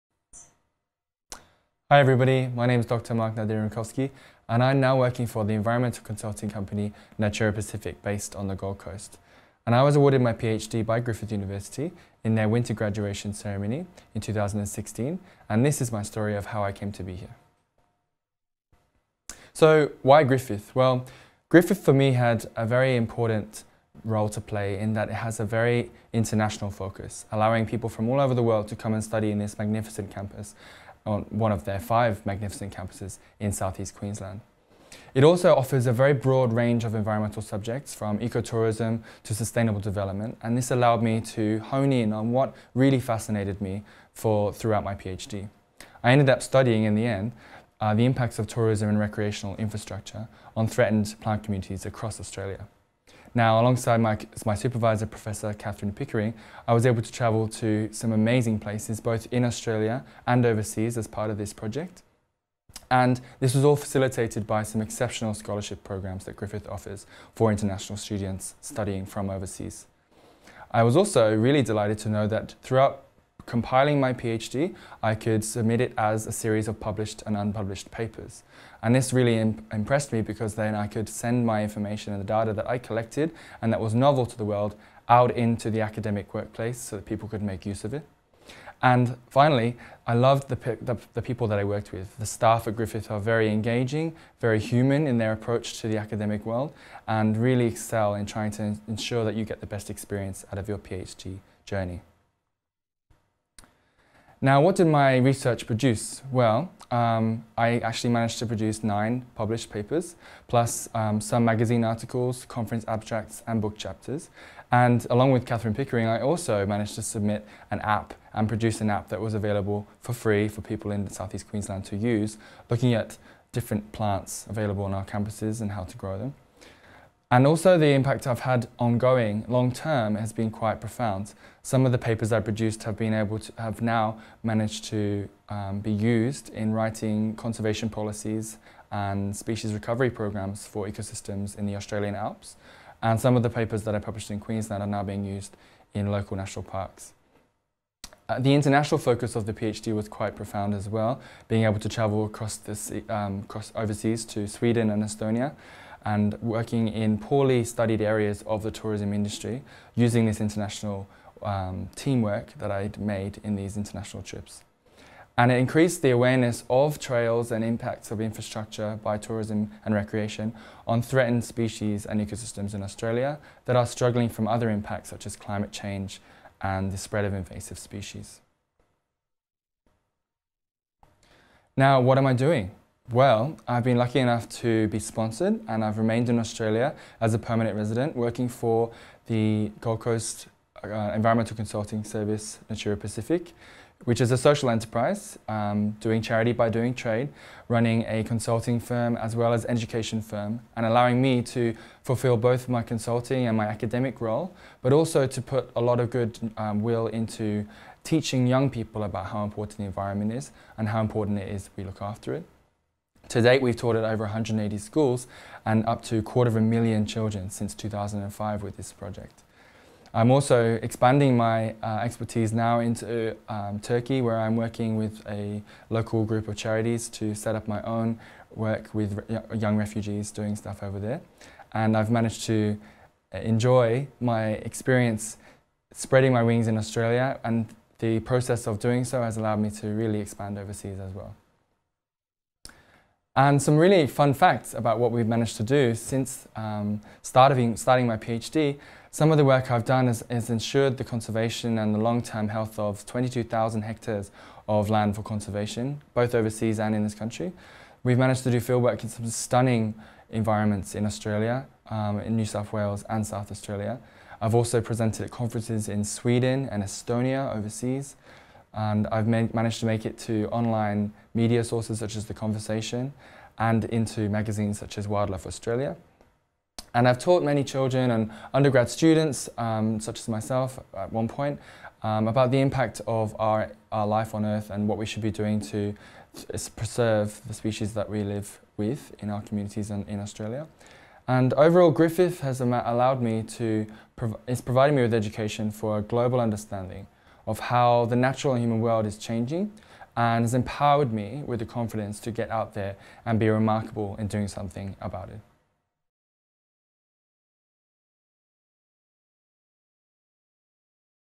PhD success story - Mark Runkowski
Our graduates keep doing amazing things. Dr Mark Runkowski talks about his experience as a PhD candidate at Griffith University in the School of Environment and what he has been doing afterwards in his new job in industry with Natura Pacific.